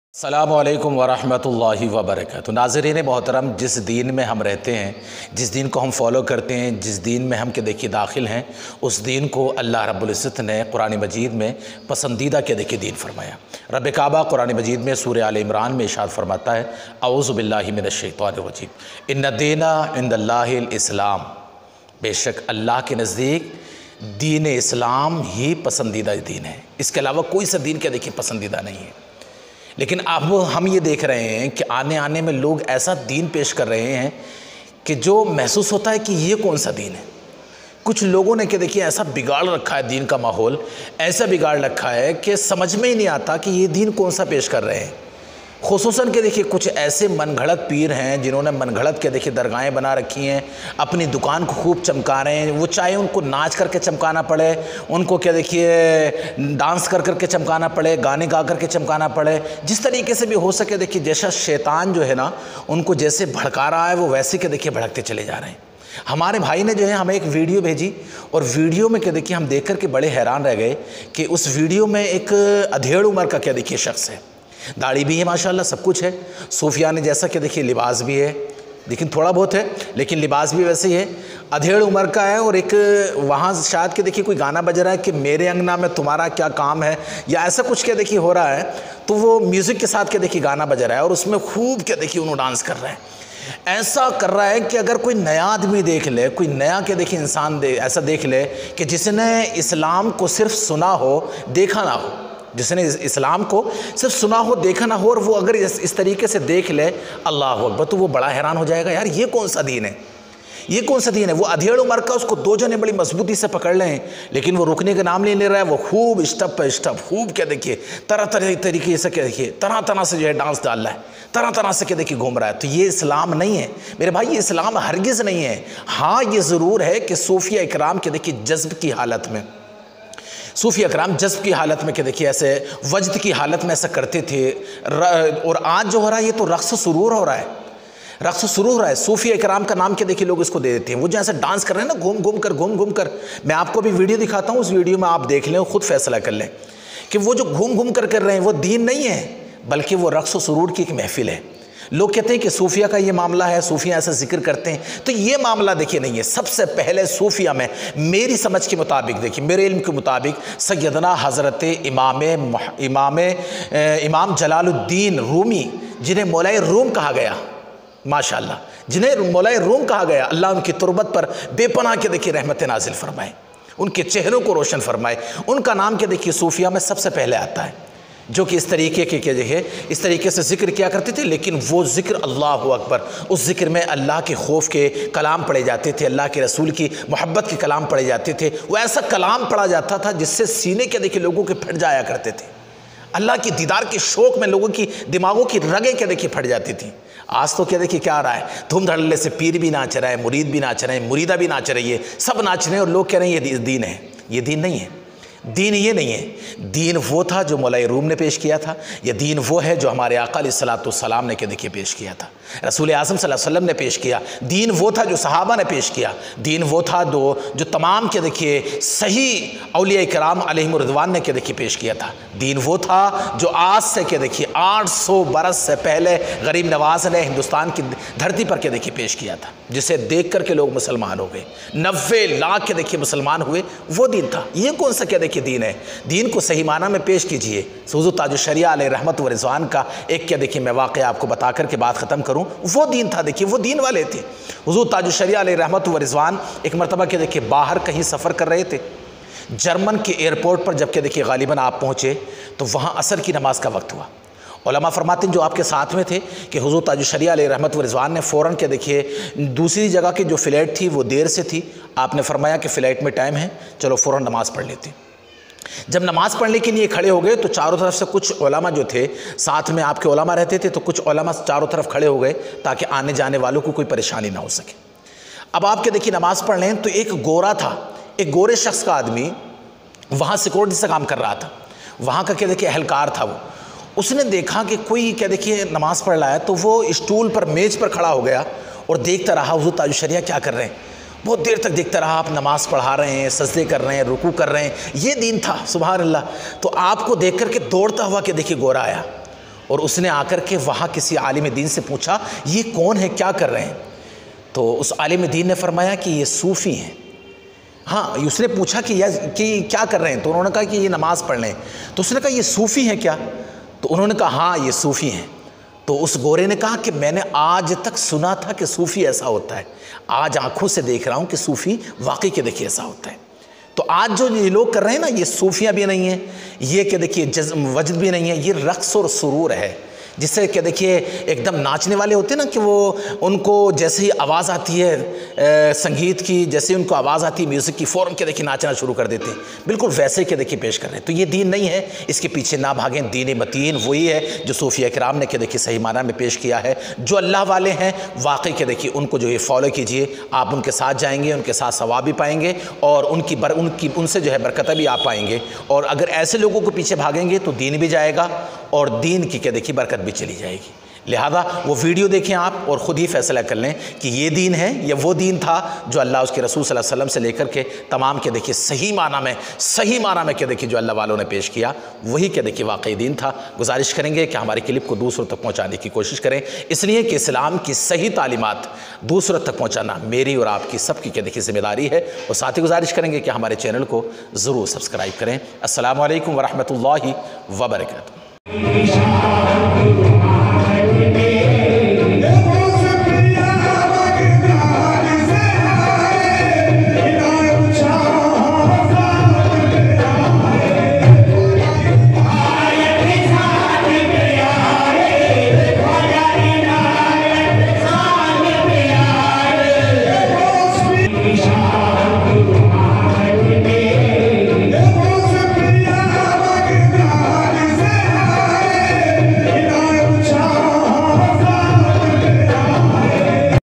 Ek Maha Baba Ke Darbar Main ।Sangeet Aur Gano Per Nachte Baba Aur Chele । Full bayan 2021
Ek Maha Baba Ke Darbar Main ।Sangeet Aur Gano Per Nachte Baba Aur Chele..
Jiske bare main Aaj Humne Poori tafseel ke sath bataya hai.